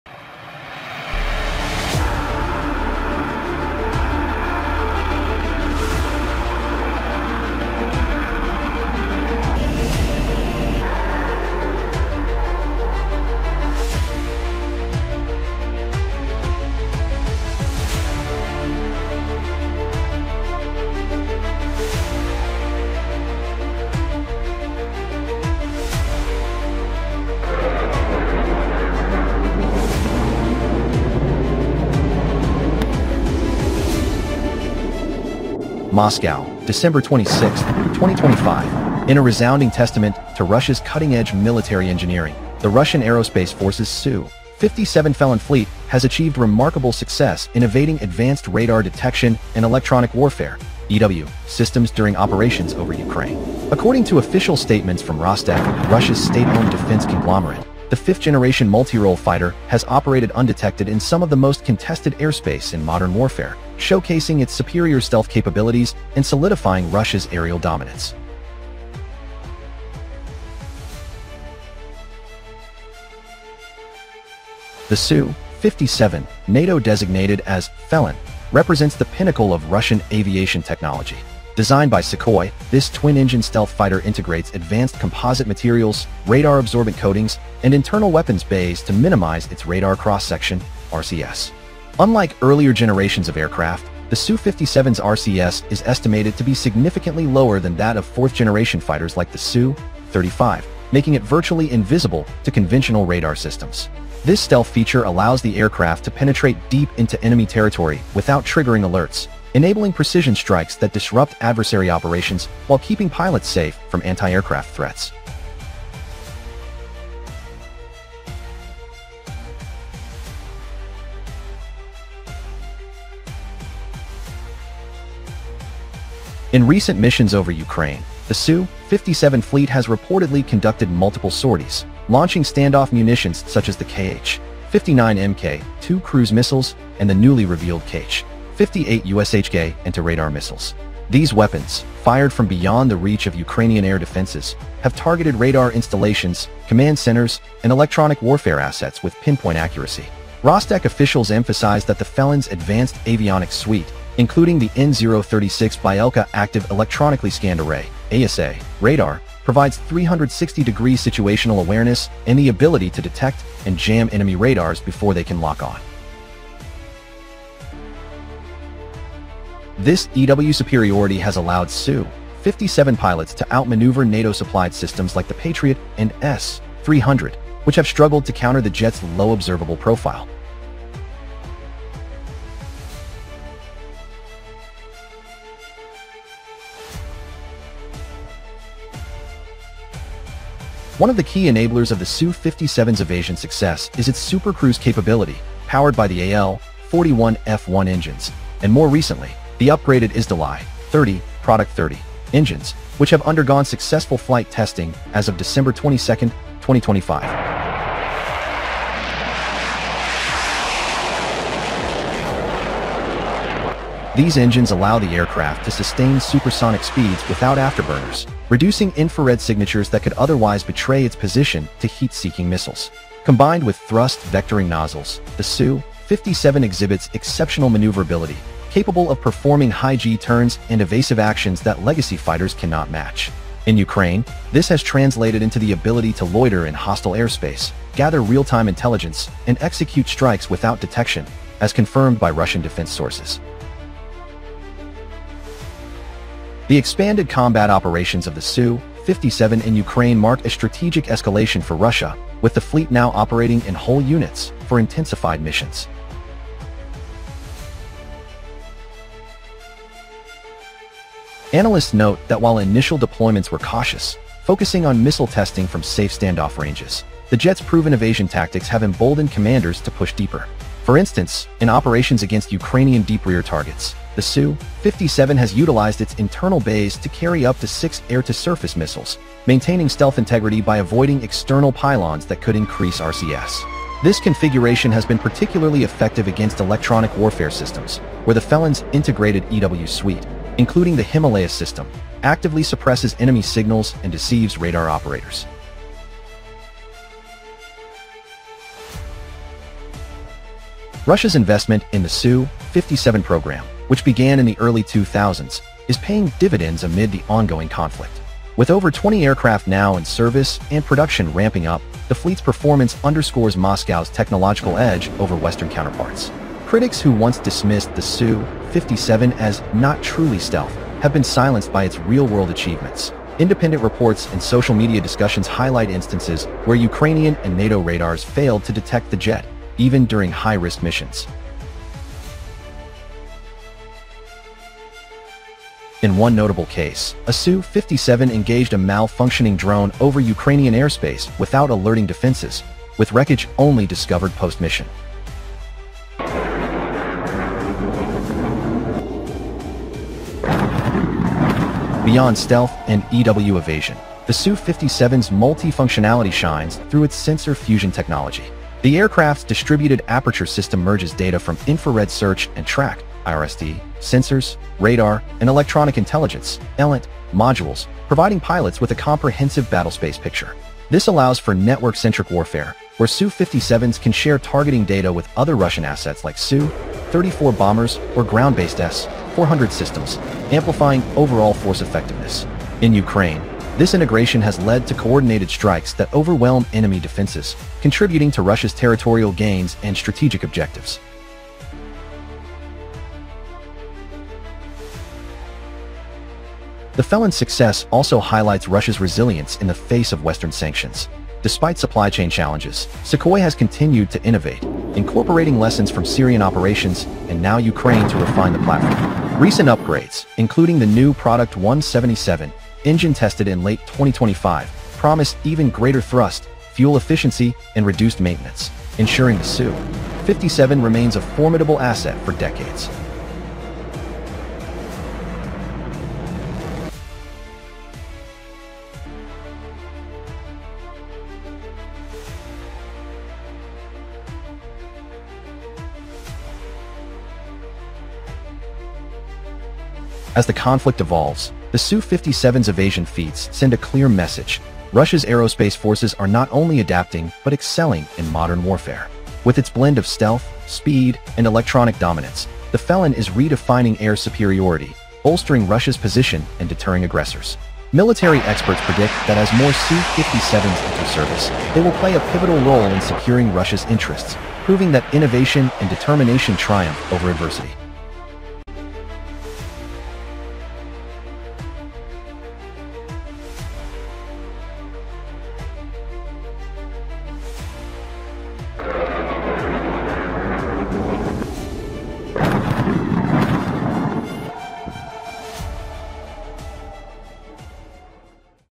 The Untouchable Su-57: Evading Radar Detection Like a Ghost in Ukraine Battlefield
Why Ukrainian Radars Fail Against Su-57 – Russia's Stealth Beast Dominates the War!